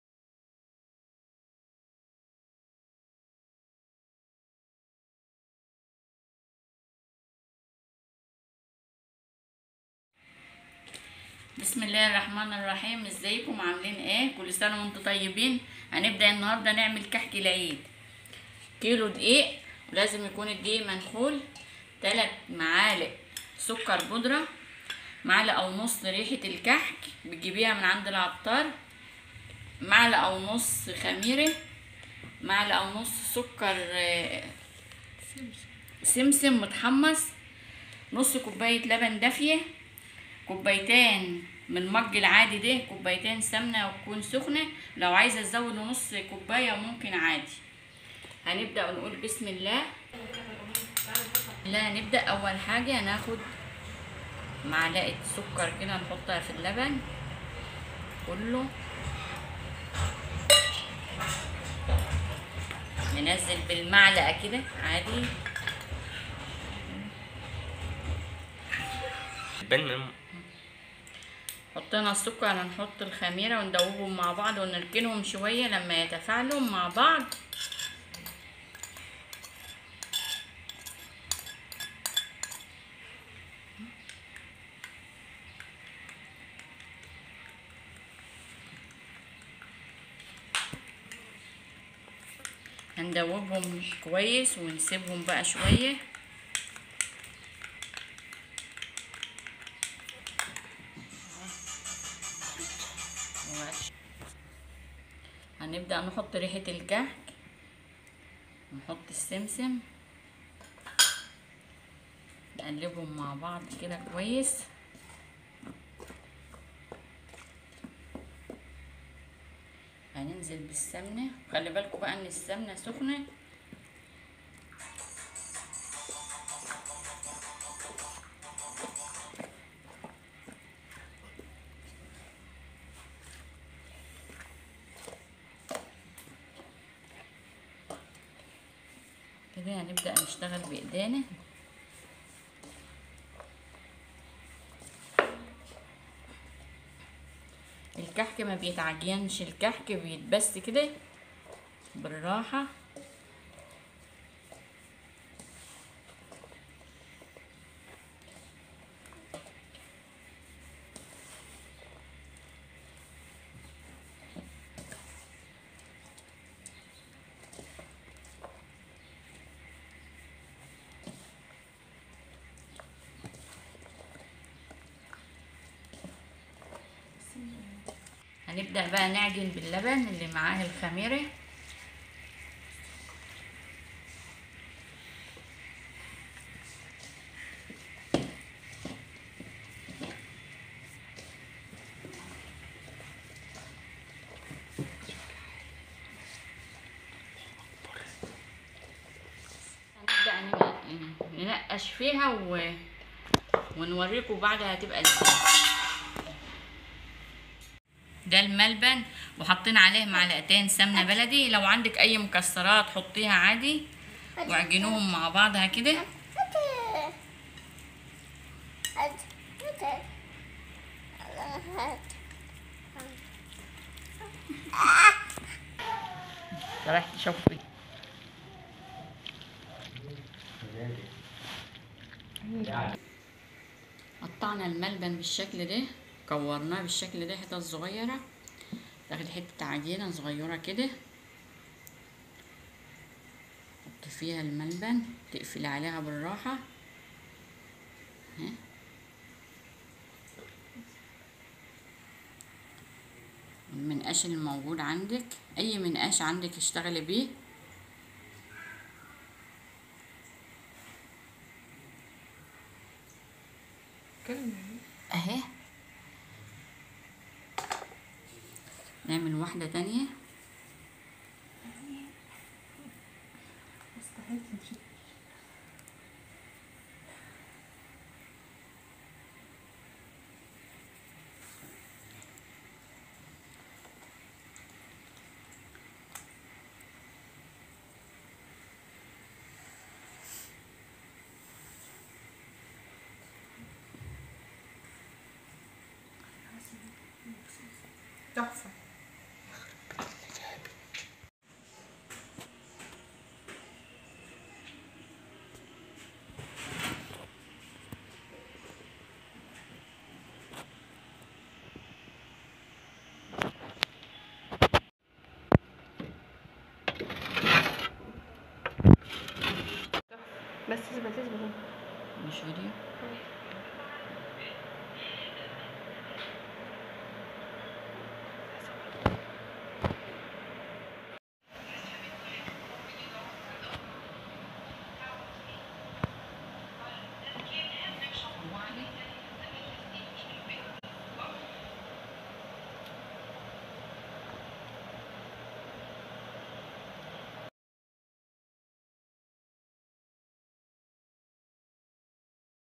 طريقه عمل كحك العيد ⁦❤️⁩
المقادير
كيلو دقيق
نص كيلو سمنه
3معالق سكر بودره
معلقه ونص خميره
معلقه ونص سمسم
ملبن الحشو 🍪